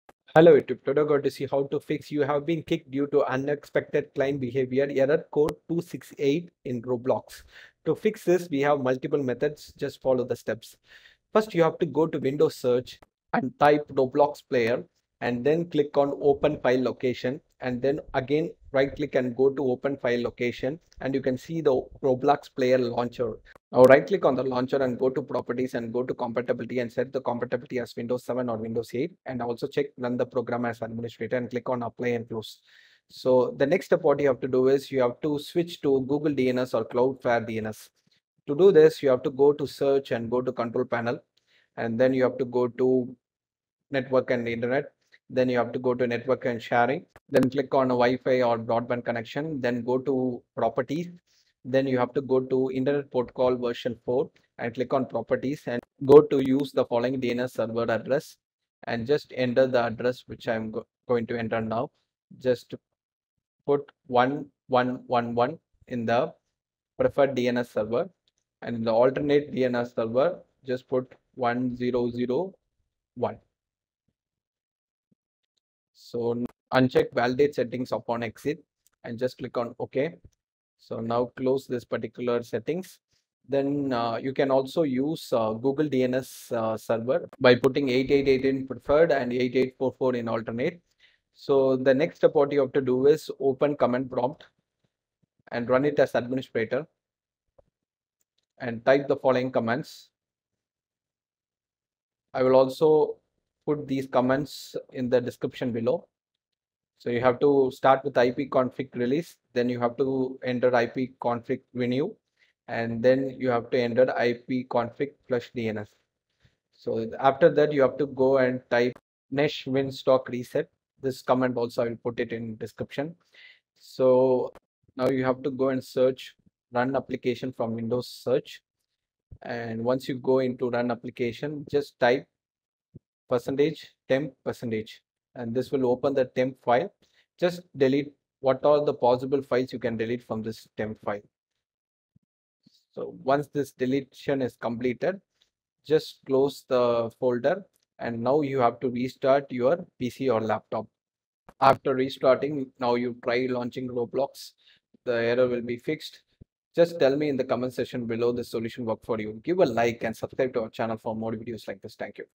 Error Code 268 Roblox Disconnected - You Have Been Kicked Due To Unexpected Client Behavior Fix
This video shows How to fix Error Code 268 Roblox Kicked Out Of Server Disconnected You Have Been Kicked Due To Unexpected Client Behavior 2021 Error Problem solved

ipconfig /release
ipconfig /renew
ipconfig /flushdns